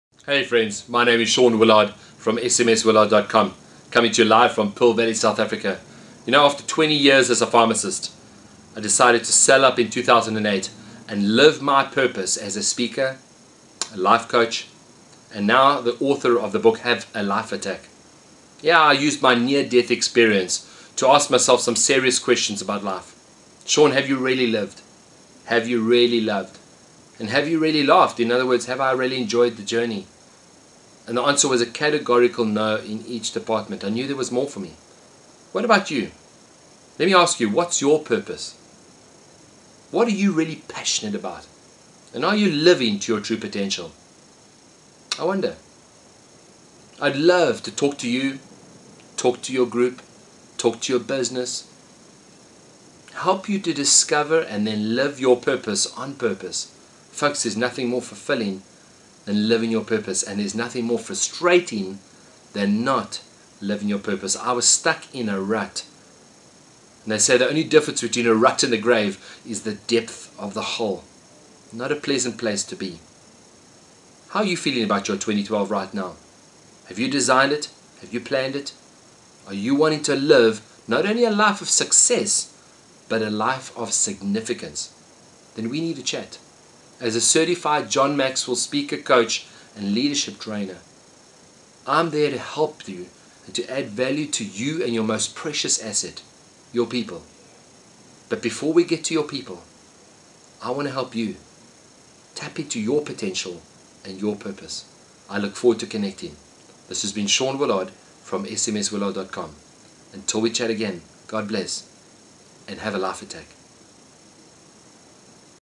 Sean Willard - Speaker, Coach & Author ~ Let's Connect
After 20 years as a Pharmacist, I sold up to Live my Purpose as a Speaker, Coach and now Author of the book Have a Life Attack.
As a John Maxwell Speaker, Coach & Trainer, I would love to connect and add value to you and your business.